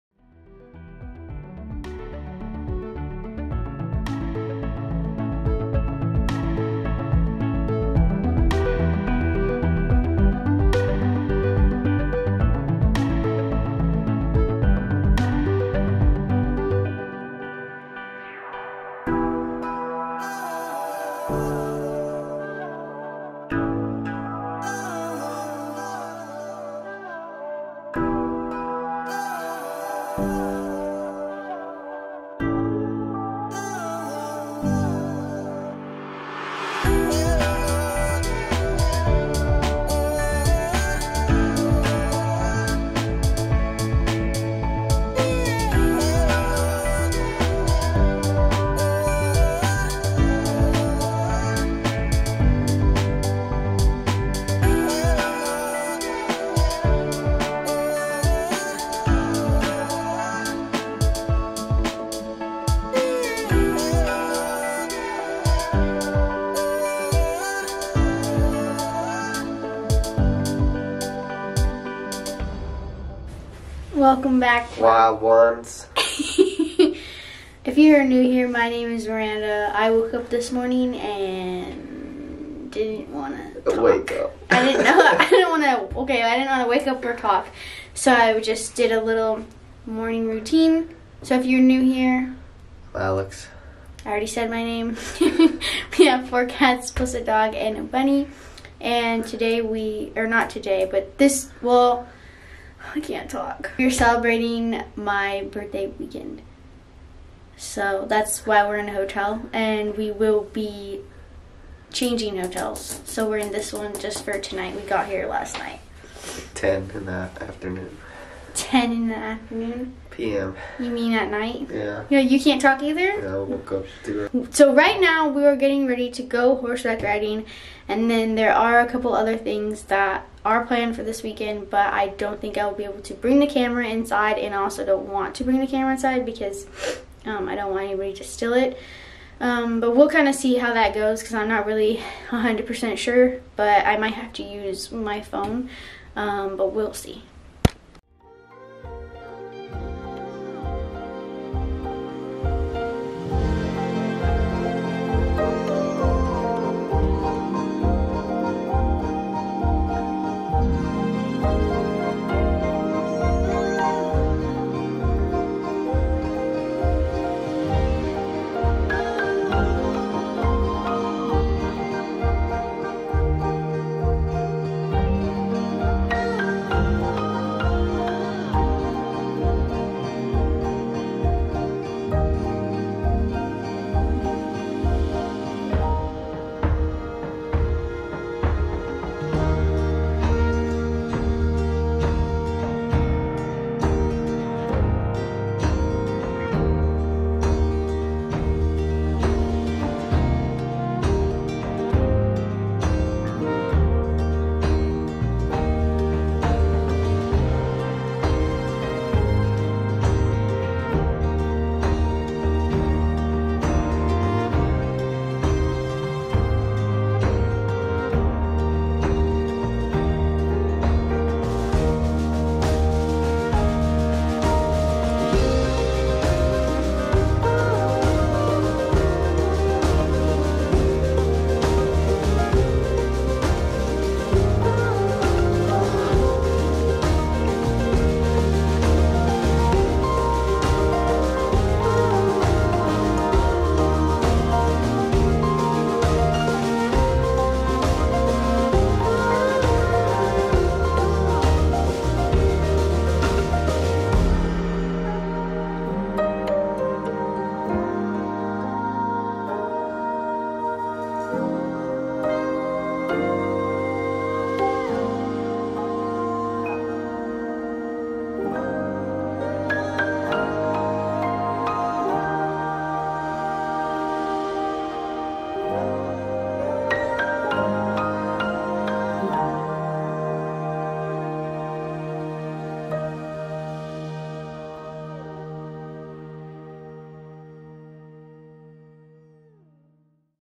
IT'S MY BIRTHDAY !
This video may be short, but it's simple and sweet. ENJOY !

Cats names :
Sara - b
Sapphire
Hope
Binx
Howlite - Angel

Dogo: Delilah
Bunny: Hazel